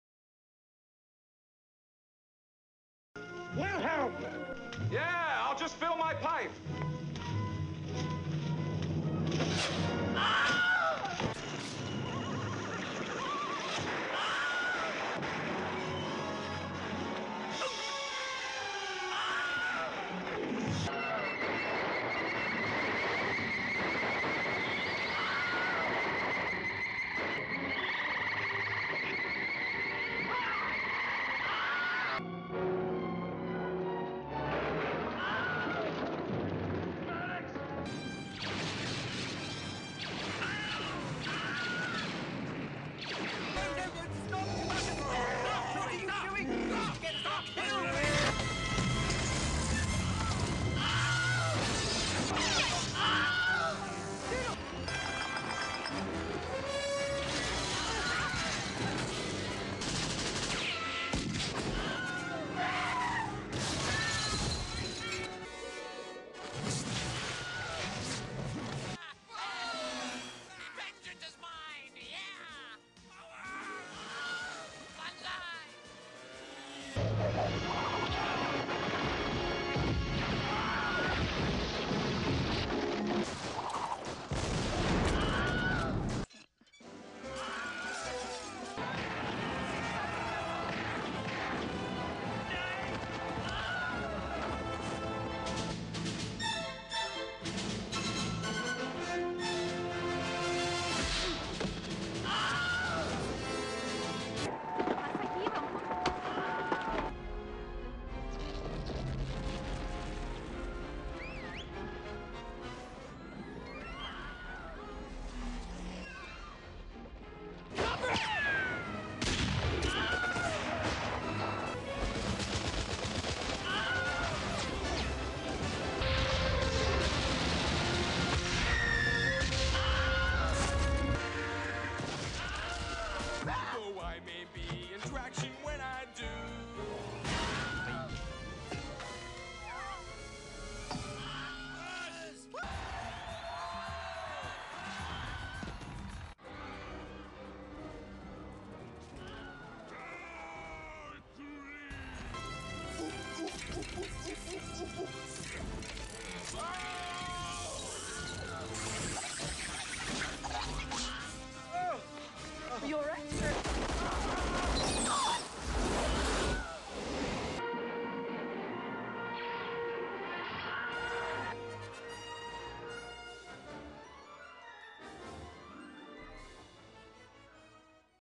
Wilhelm Scream - Der Schrei aus der Retorte
Ein Schrei, der für sehr viele Filme immer wieder genutzt wird. Wozu neu aufnehmen, wenn man's im Archiv hat?